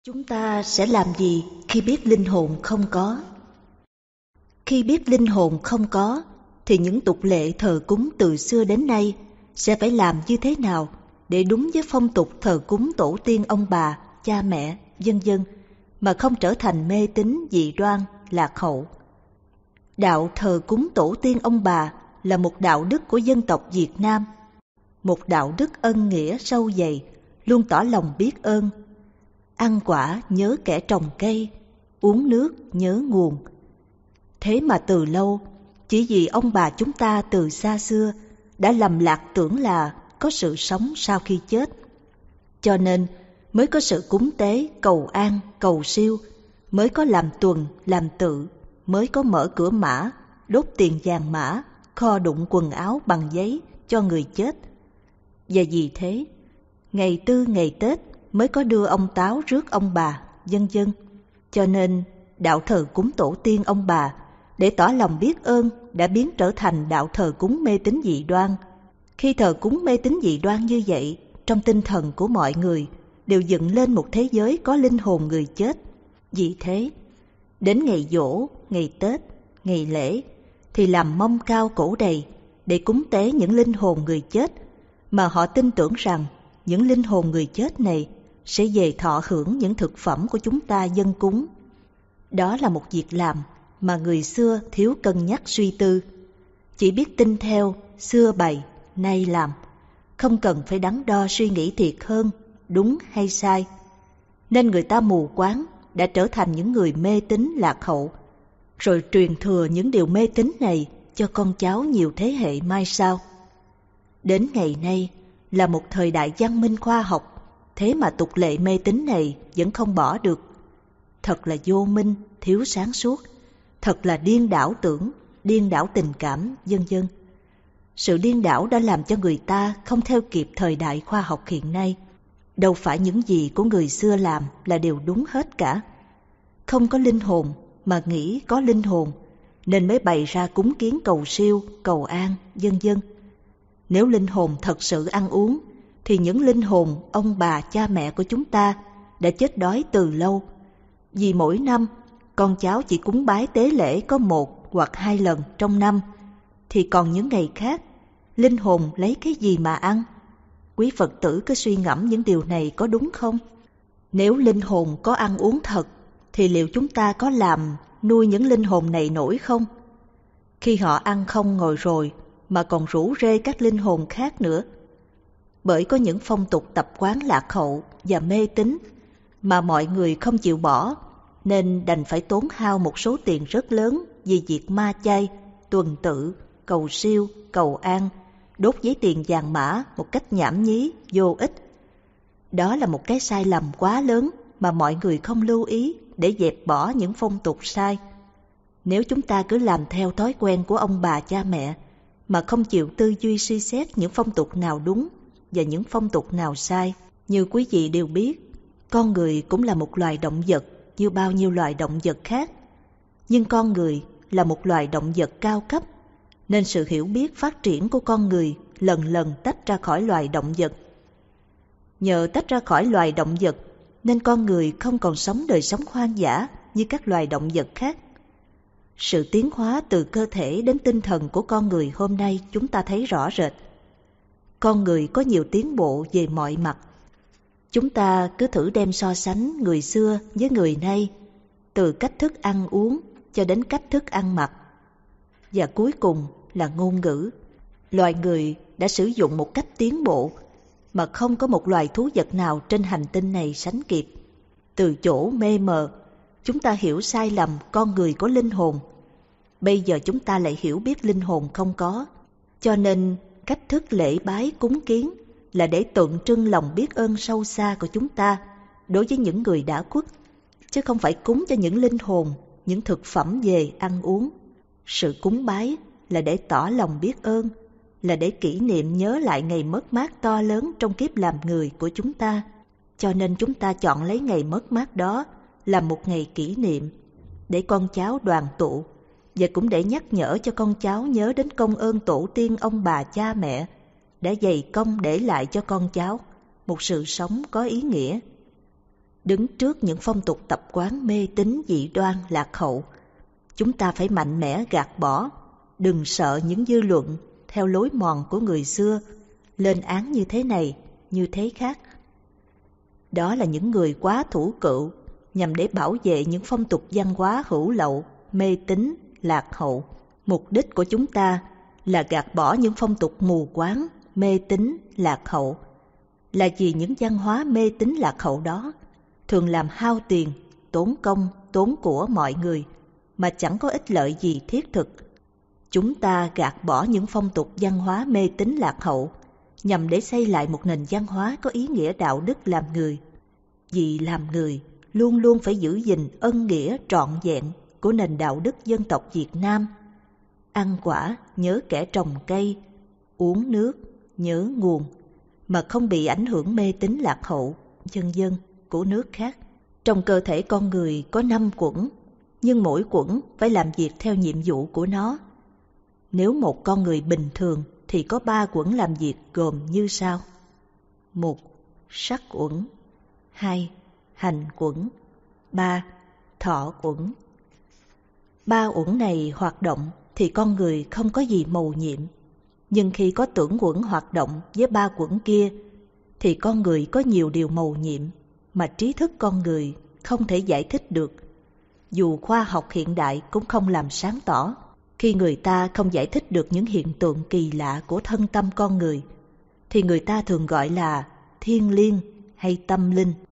Có nên cúng tế linh đình mời ông bà về ăn giỗ? Alahan Thông Lạc giảng.
Cúng tế linh đình mời ông bà về ăn giỗ là mê tín, lạc hậu. Ngày giỗ chỉ nên coi là ngày tưởng niệm, tưởng nhớ công ơn ông bà, chứ không có linh hồn ông bà về hưởng những thức ăn mà con cháu dọn lên cúng bái.